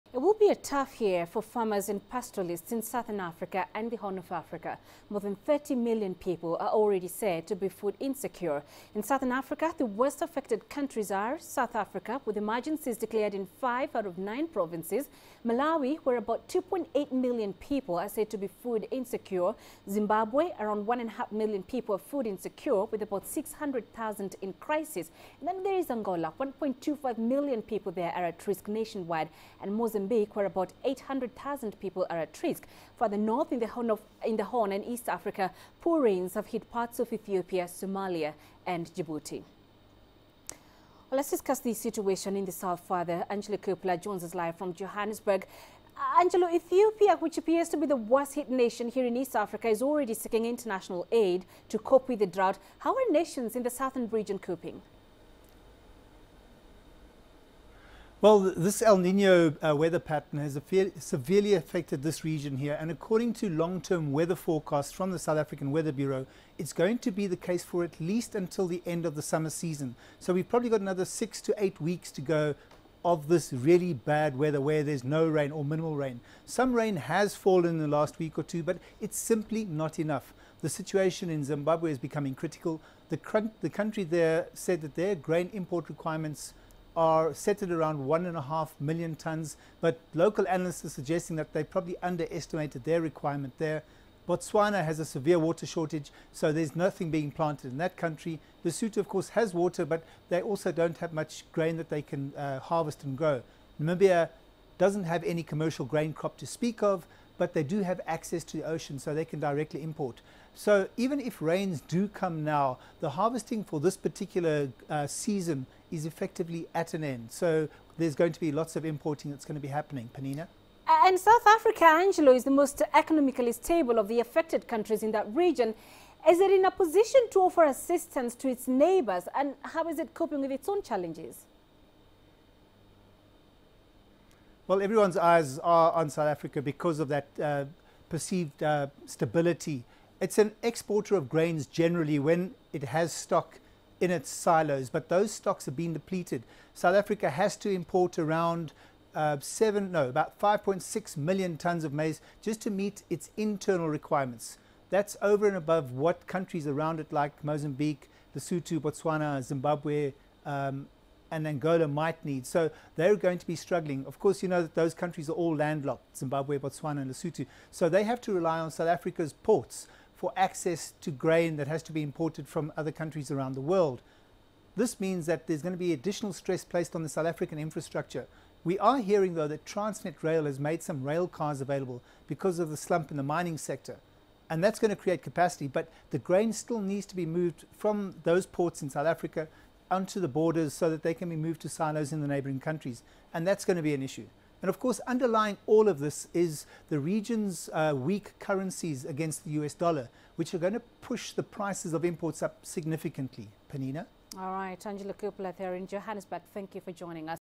Ethiopia seeking international aid to curb effects of drought
Ethiopia is calling for urgent international support for millions facing food shortages. This follows a devastating drought attributed to the El-Nino weather phenomenon. Ethiopia currently needs over a billion dollars for relief but donors have so far only raised about 30 percent of that.